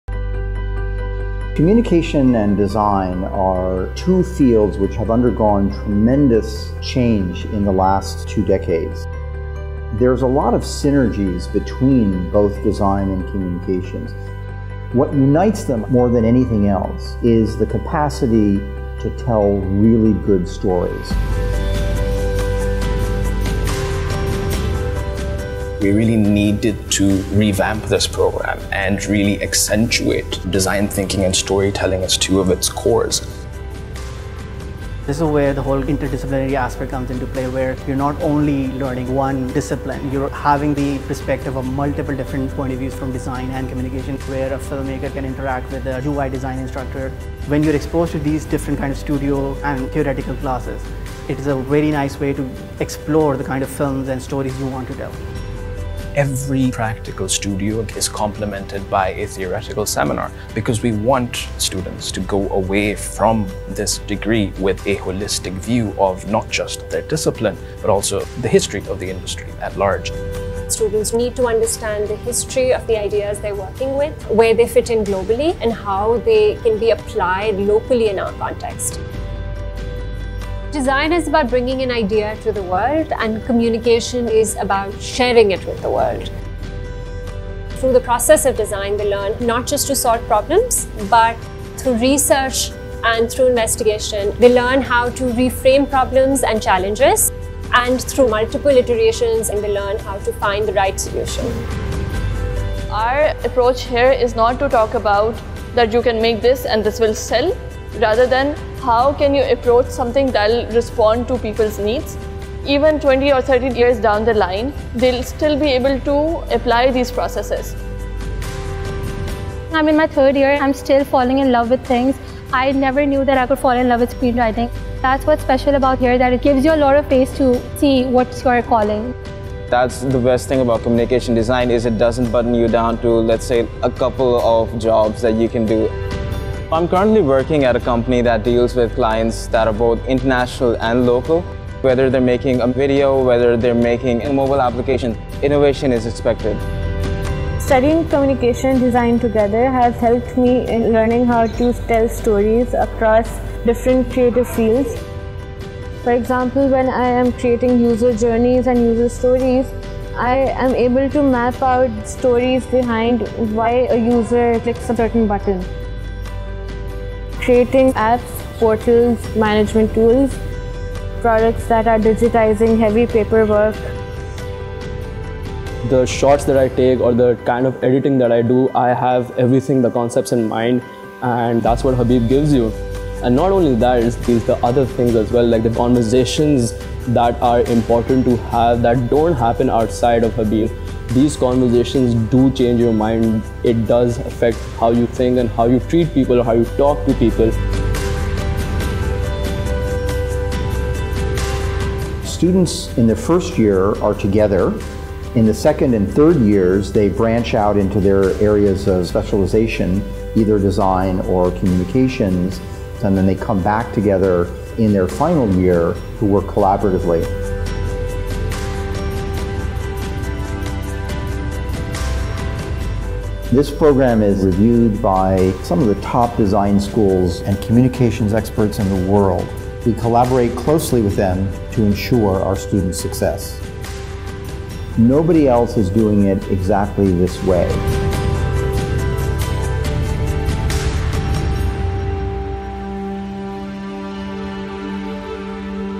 Habib University's Communication and Design Program
Habib University is proud to introduce its revamped Communication and Design program.

It is a robust, internationally peer-reviewed, 4-year bachelors degree program that brings together the two complementary yet creative worlds of communication and design together in a unique manner that provides students with holistic learning of the two disciplines, transforming them into Changemakers who are in sync with the emerging needs of the market.

The second round of admissions at Habib University is open, APPLY NOW